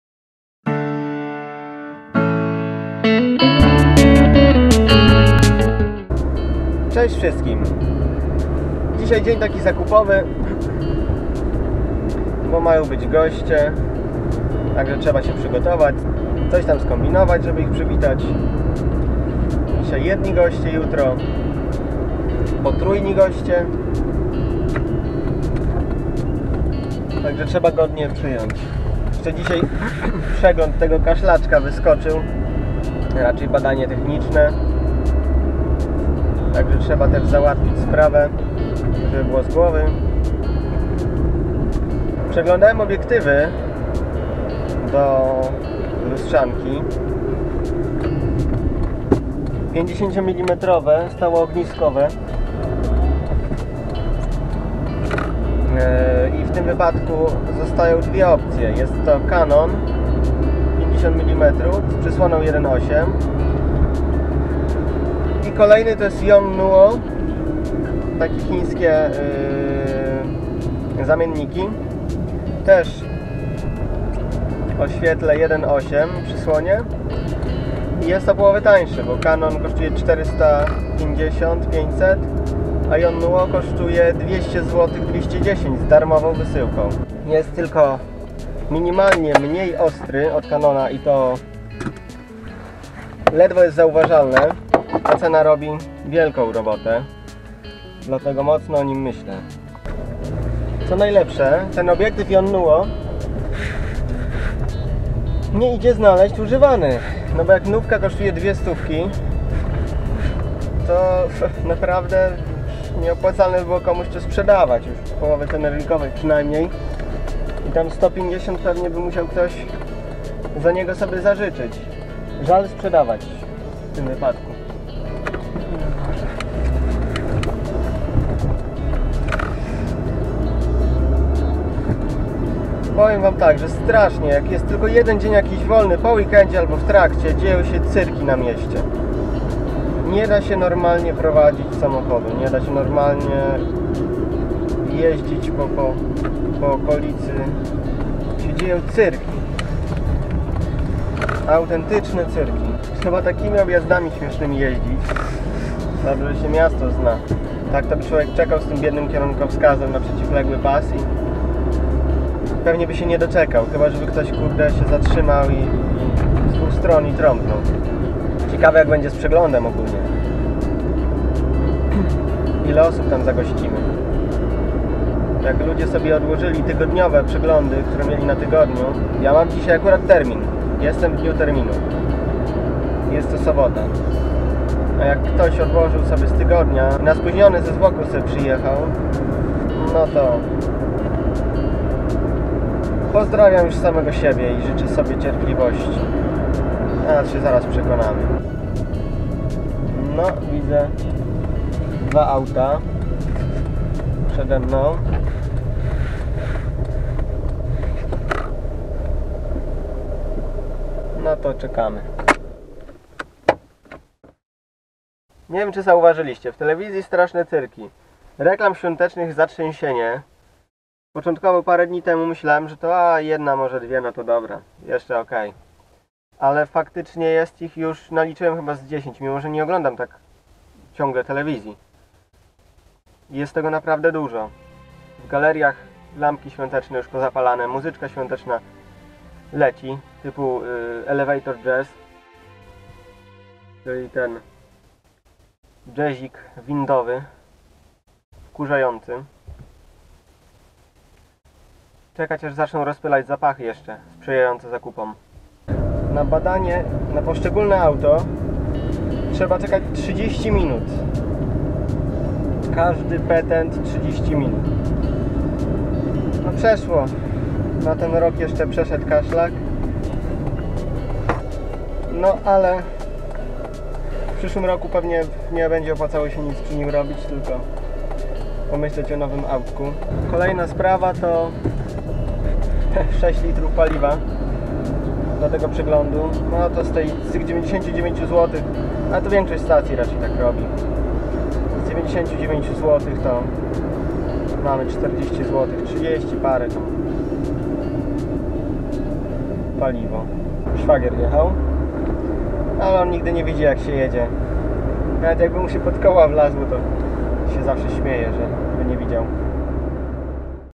BananVloguje - Jeden dzień wolnego, zapasy jak na apokalipsę [DAILY#25]
Zapraszam co dzień!!